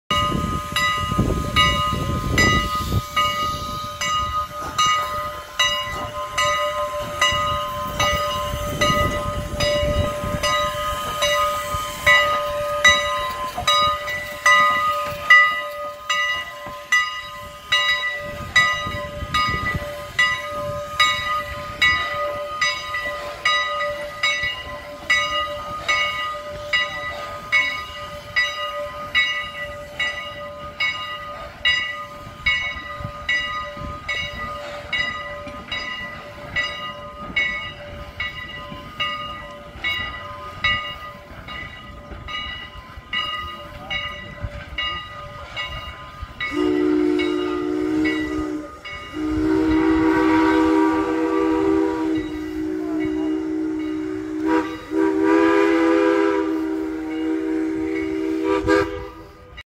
Strasburg Railroad #89
After taking on water after a days work #89 heads off to the shed for the night. 4/7/19